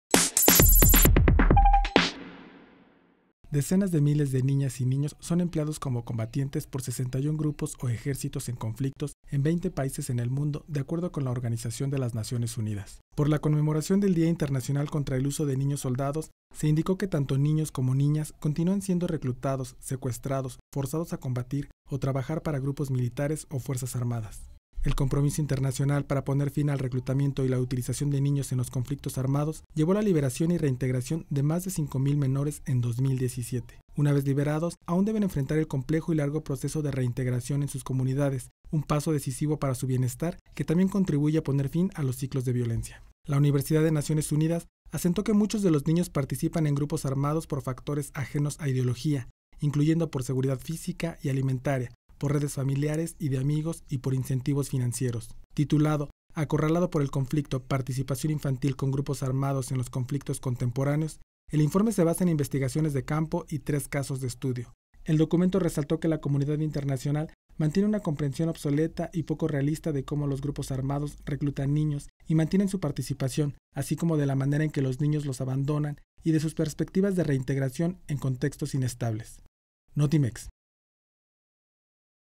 Decenas de miles de niños soldados combaten en el mundo: ONU
Por la conmemoración del Día Internacional contra el Uso de Niños Soldados, se indicó que tanto niños como niñas continúan siendo reclutados, secuestrados, forzados a combatir o trabajar para
grupos militares o fuerzas armadas